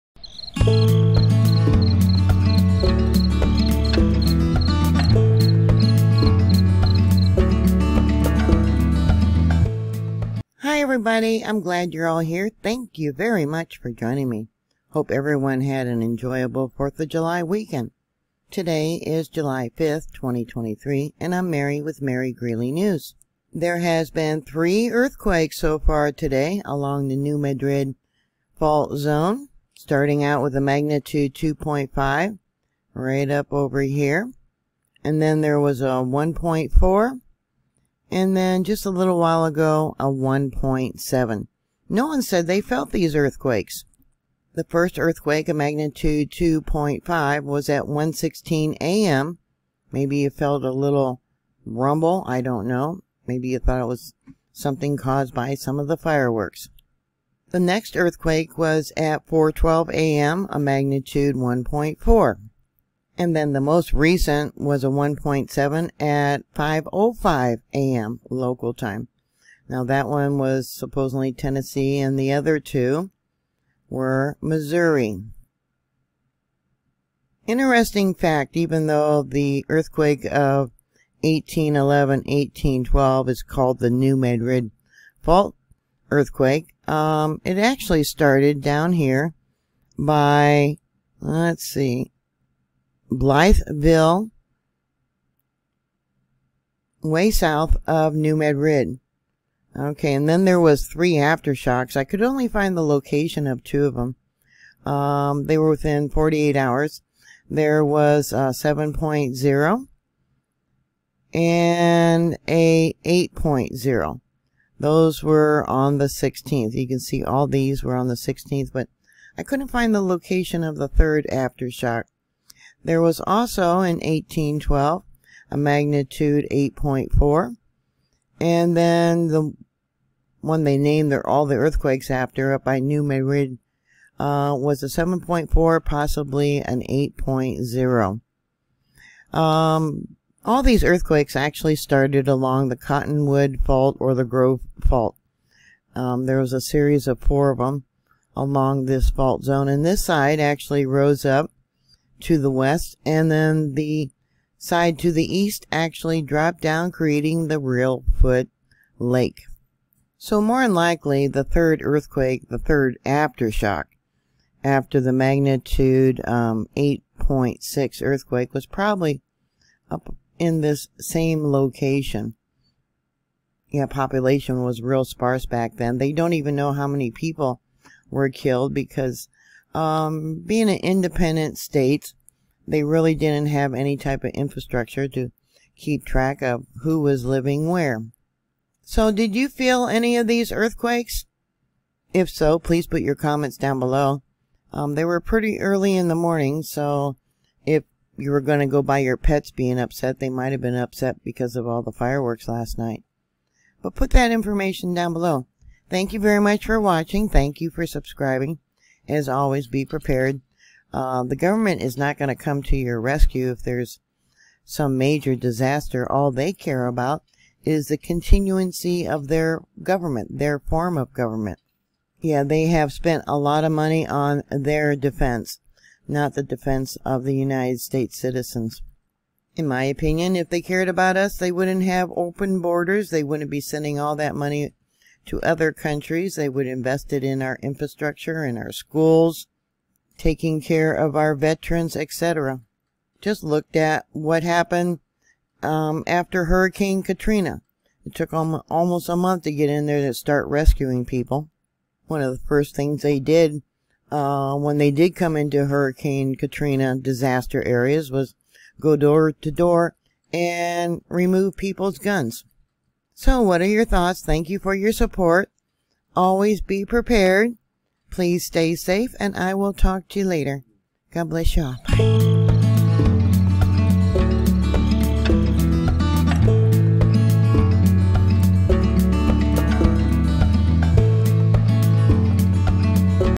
New Madrid Earthquake History, Latest Earthquakes, 3 Today
The past earthquake were not what you were taught. The first earthquake wasn't even in the town of New Madrid. Today there was 3 quakes so far today the larges being a M 2.5